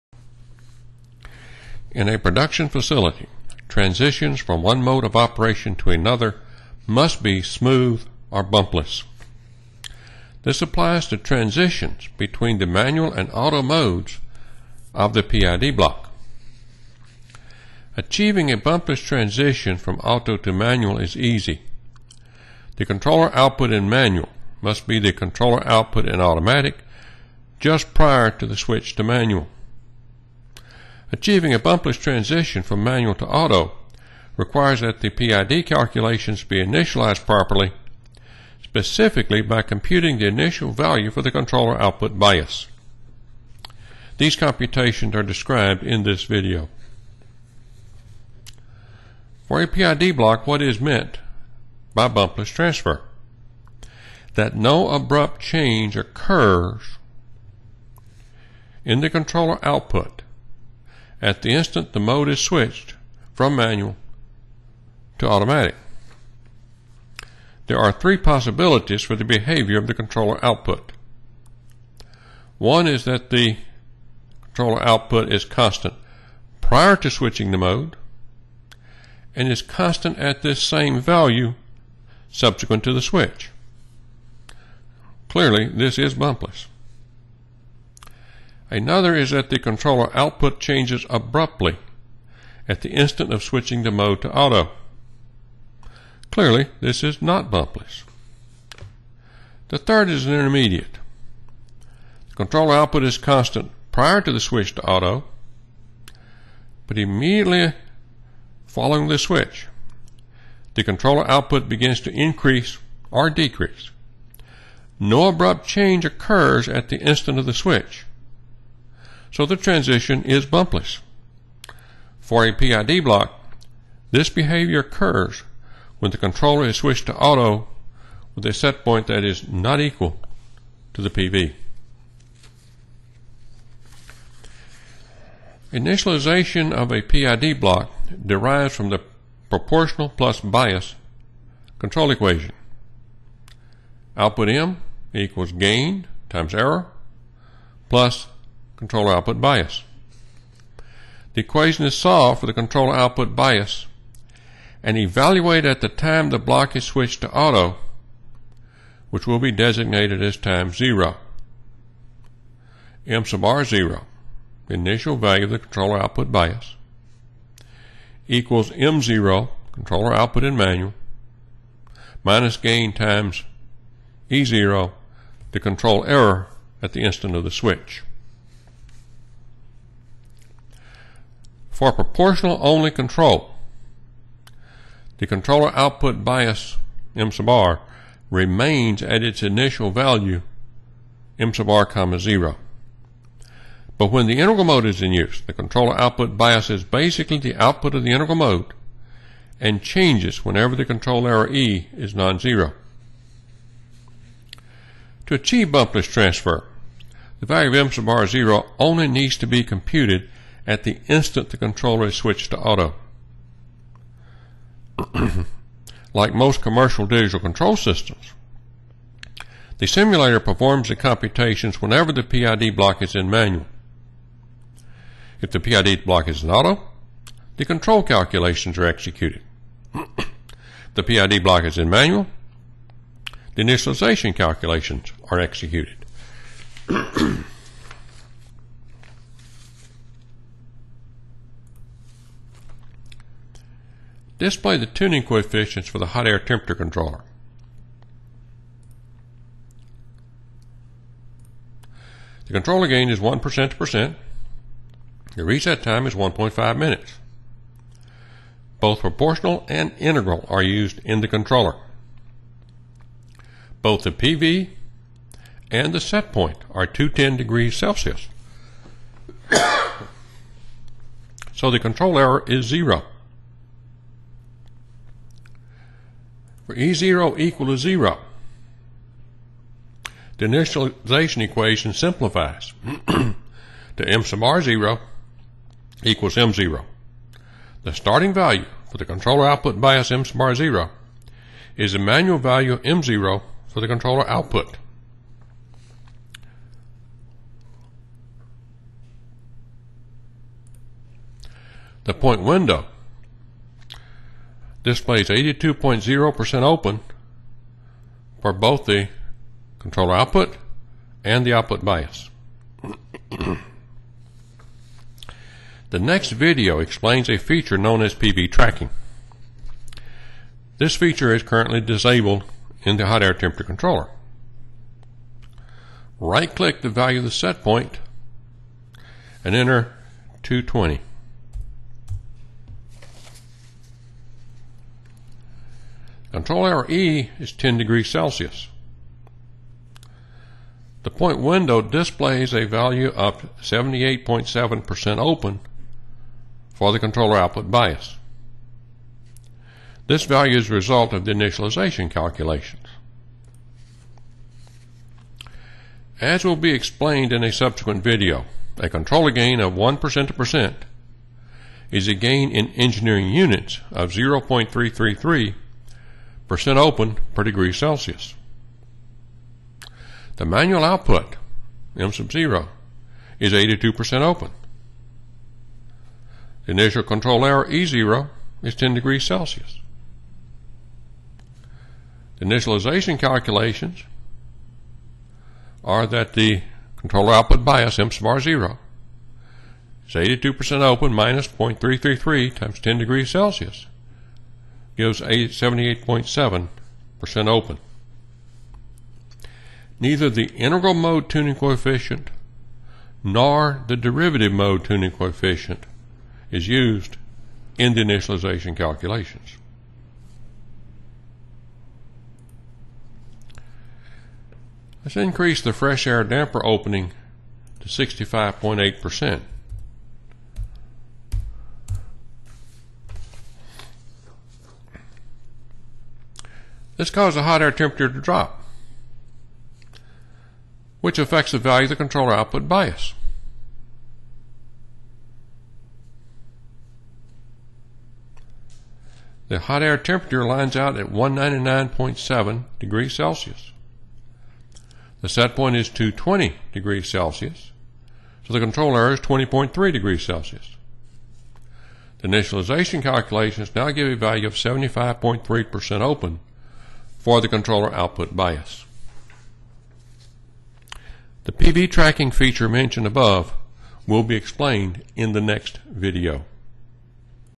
Bumpless Transfer
Achieving a bumpless transition from auto to manual is easy – the controller output in manual must be the controller output in automatic just prior to the switch to manual.  Achieving a bumpless transition from manual to auto requires that the computations be initialized properly, specifically, by computing the initial value for the controller output bias.  The computations are described in this video.